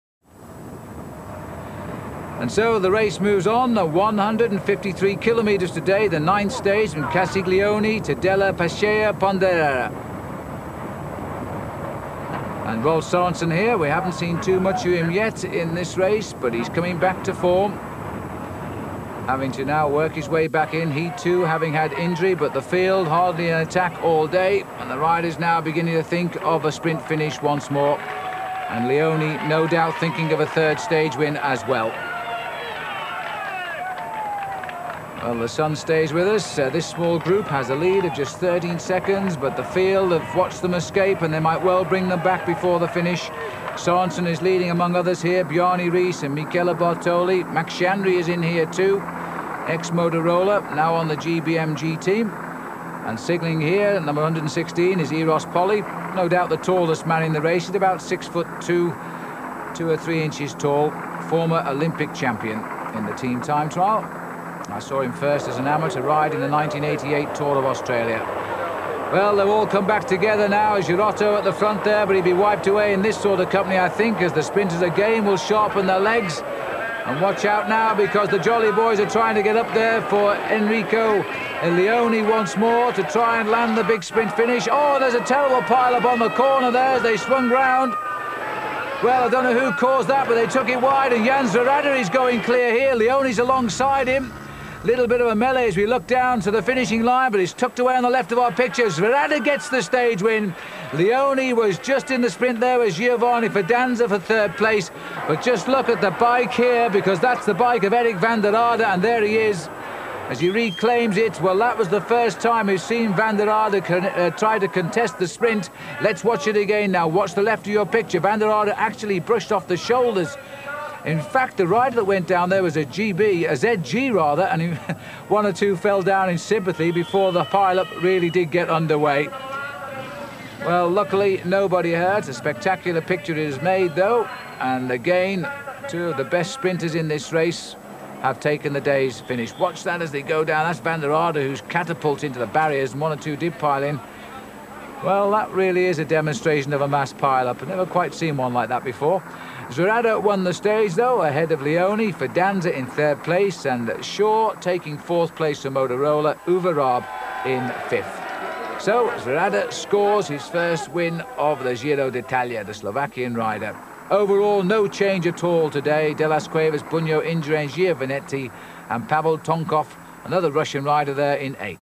Video tappa giro d'Italia 1994 a Pontedera
L'arrivo della tappa del Giro d'Italia 1994 a Pontedera